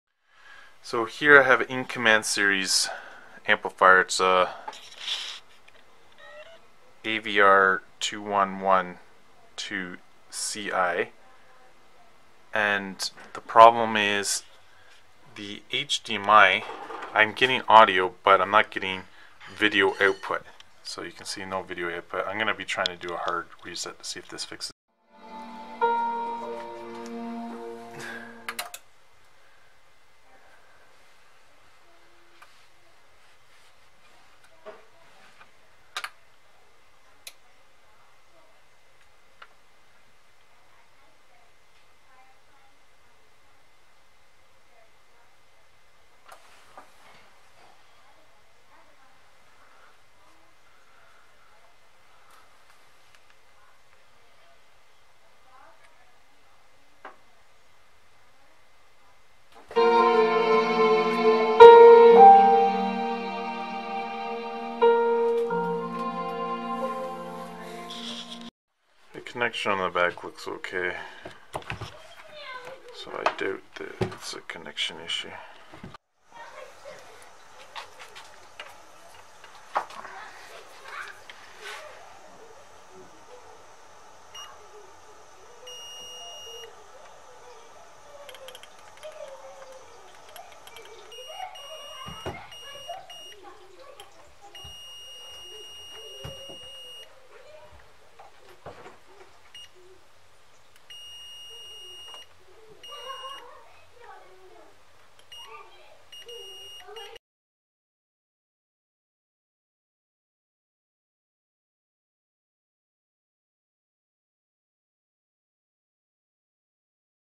AVR video not working? Check this out. IN-Command diagnostic (Model VR 2112CI)
This AVR has audio but the hdmi video output does not work. I checked the hdmi connection and did a full reset but it still does not work. The issue I believe is the hdmi chip but to replace this chip would require a lot of heat and potential damage to the pcb board.